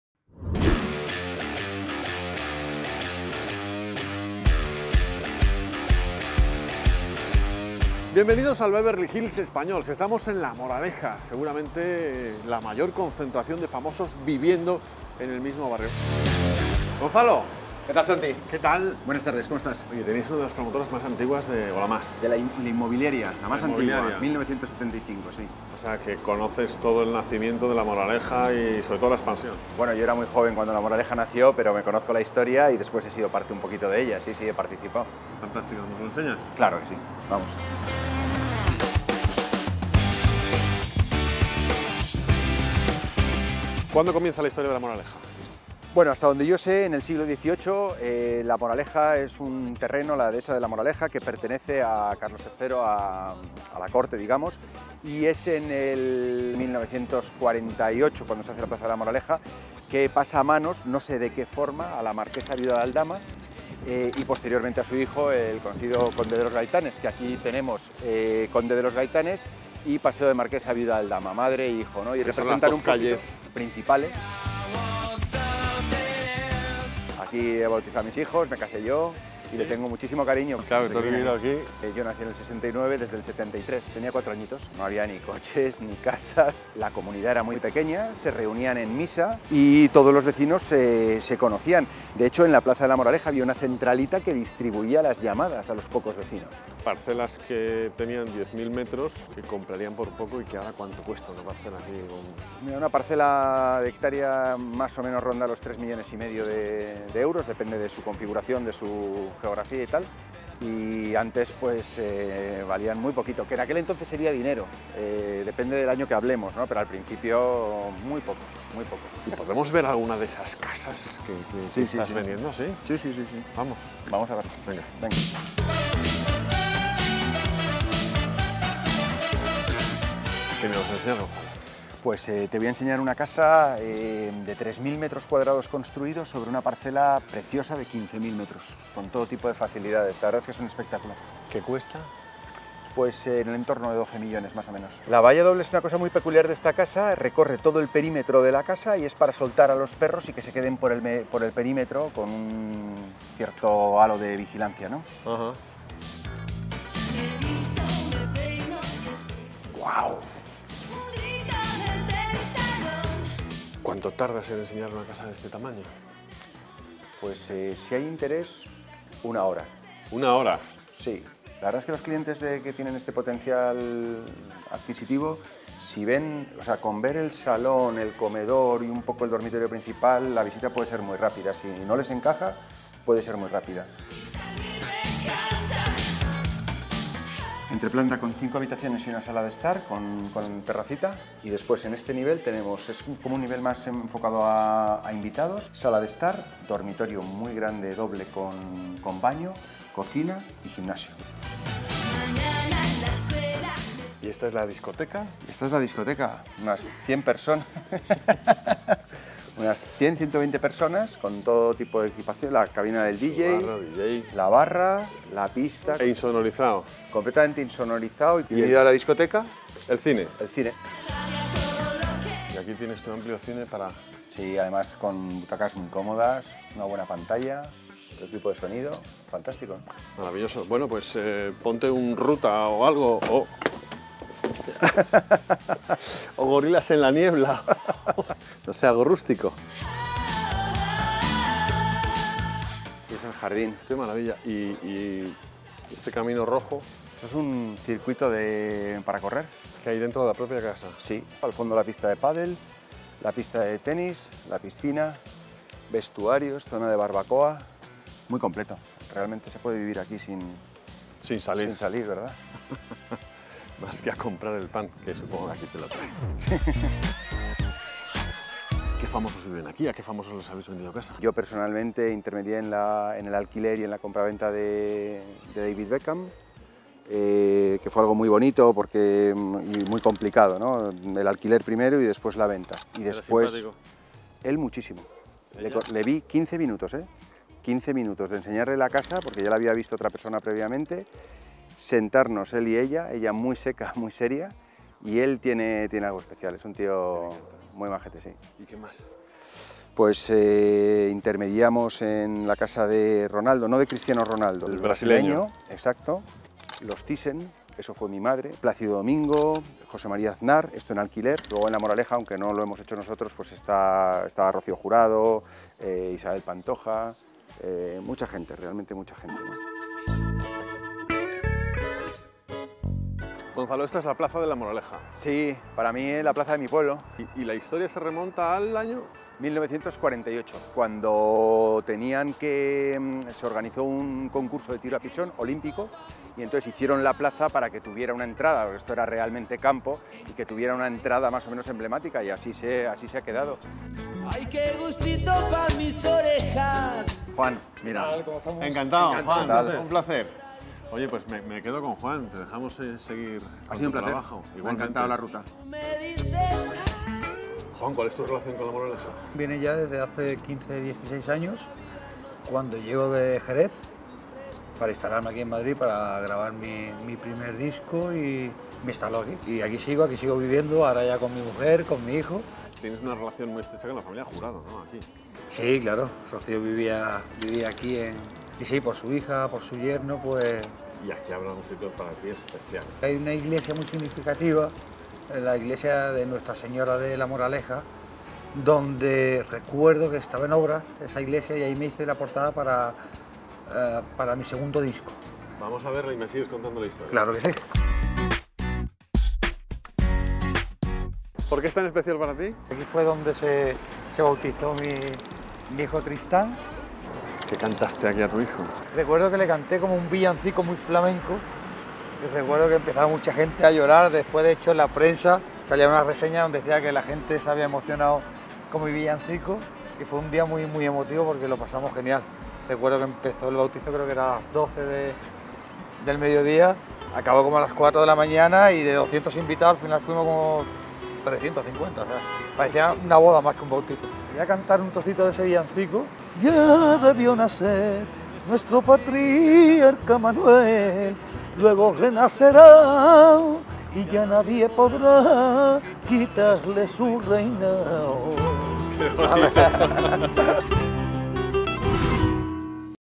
La Moraleja, el barrio más caro de Alcobendas
Los inicios de La Moraleja se remontan a principios del siglo XVIII. Entonces, formaba parte de los aledaños de El Pardo, de Patrimonio Rreal. Carlos III lo incluyó entre sus cuarteles denominándola «Dehesa de la Moraleja», situada en el término de Hortaleza, hoy distrito de Madrid, pero hace años pueblo independiente.

En 1946 se desarrolló el plan de ordenación de La Moraleja como conjunto residencial de ciudad jardín. Se trata de un núcleo naturalmente preservado y distinto de su entorno, en el que más del 70% es superficie verde, con valiosas características originales de bosque mediterráneo. El número de habitantes es de unos 12.000 y se considera que cuenta con la mayor concentración de chalets de toda España. Lola Flores, Rocío Jurado, Tita Cervera o David Beckham han sido algunos de los famosos que han vivido durante mucho tiempo en La Moraleja. Conocemos la zona de la mano de Gonzalo, cuya familia tuvo la primera inmobiliaria que se instaló en La Moraleja, y visitamos una de las casas que se venden. ¿El precio? nada más y nada menos que 12 millones de euros.